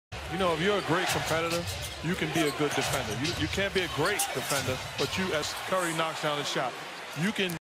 NBA, playoff 2019, Warriors vs. Trail Blazers, Round 3, Game 2, Move 20, Seth Curry, 2 pointer
Golden State Warriors - Portland Trail Blazers, Game 2, 16/05/2019, quarter 2
Seth Curry, 2 pointer